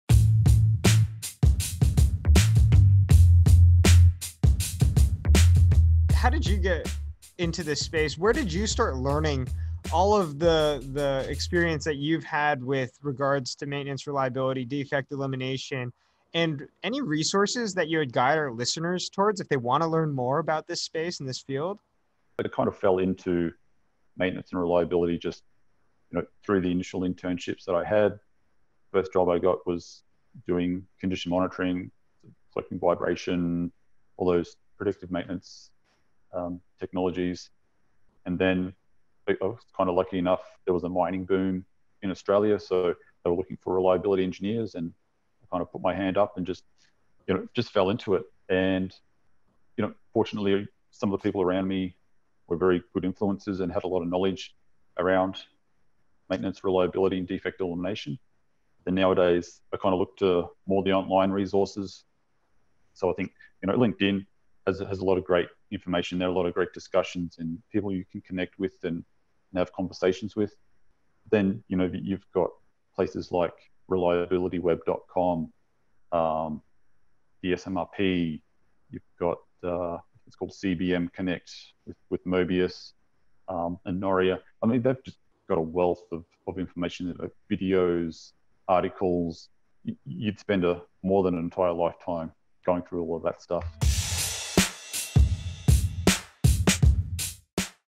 "Where do you go to learn more?" with Tim Rice
This week on Masterminds in Maintenance, we dive deeper into the topic of defect elimination! Tim Rice shares with us the big impact defect elimination has on the maintenance and reliability industry, and his experience in this field!